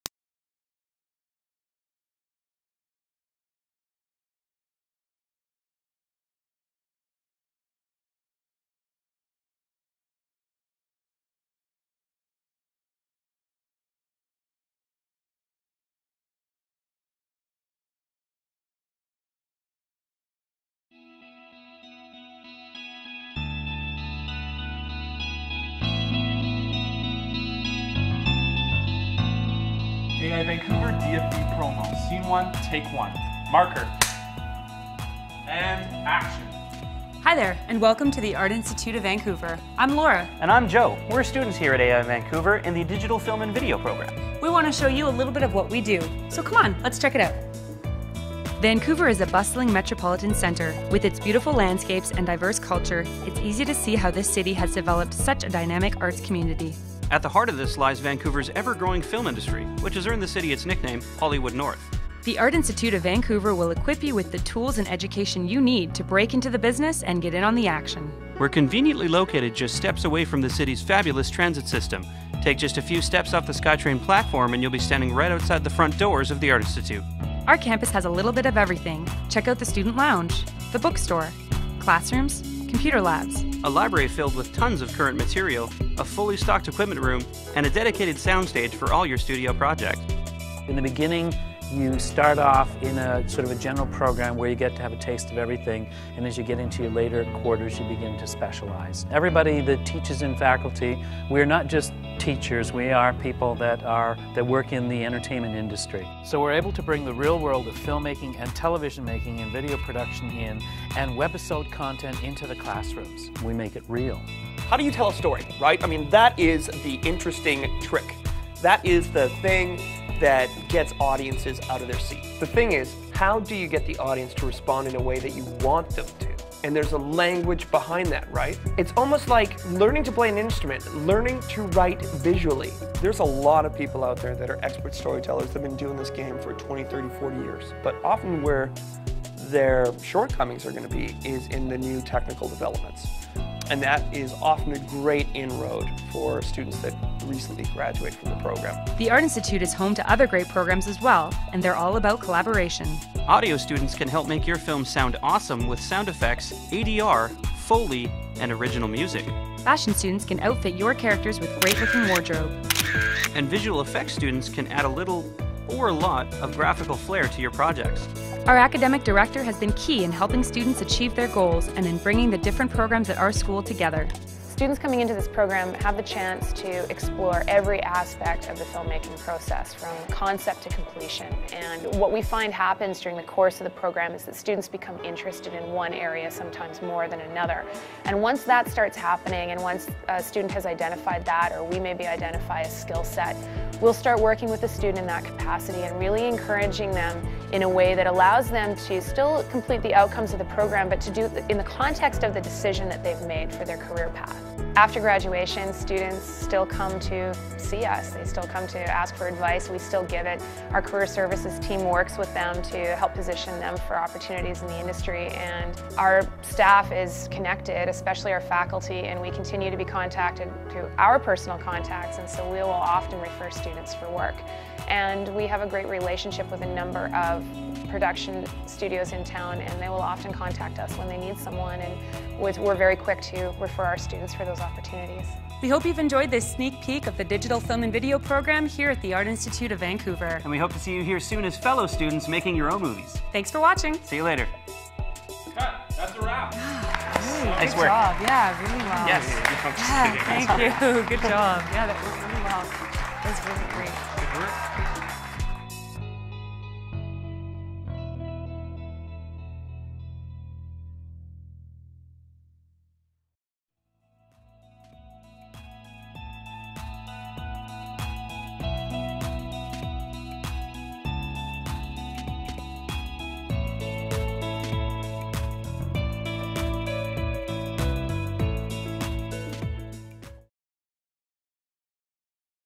Estudia Vídeo y Cine Digital en el Art Institute of Vancouver
Recuerda que estudiar otro idioma en el extranjero no es la única opción a tu alcance, también puedes tomar cursos especializados como este programa en Vídeo y Cine Digital que ofrece el Art Institute of Vancouver. ¡Especialízate en el extranjero!